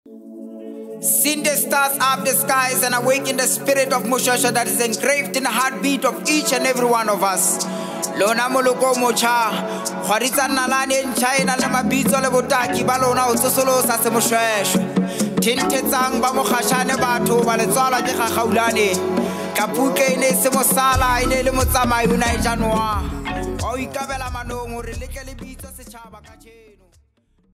Celebrating Moshoeshoe's Day 2023
This Moshoeshoe’s Day, we celebrate the wisdom & legacy of Moshoeshoe, the founder of the Basotho nation!

Today, and every day be a proud Mosotho, celebrate through dance, feasting or storytelling.

Happy Moshoeshoe’s Day.🇱🇸